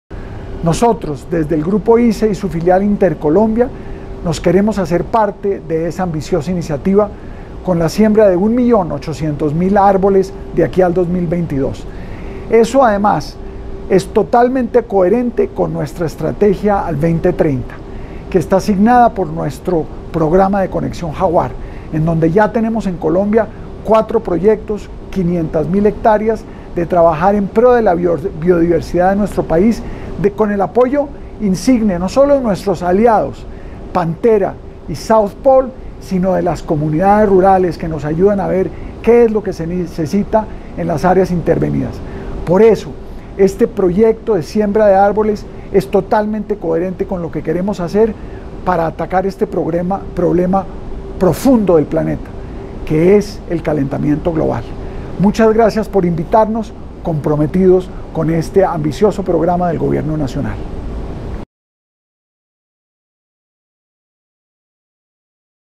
Aportaremos 1,8 millones de árboles para un mejor medio ambiente
El compromiso que asumió expresamente Grupo ISA con el Gobierno colombiano, a través del Ministerio de Medio Ambiente y Desarrollo Sostenible, busca definir una acción concreta que aporte al Plan Nacional de Desarrollo, a través de la siembra y el mantenimiento de estos árboles que generarán mejores condiciones ambientales en zonas de interés ecológico.

Conéctate con ISA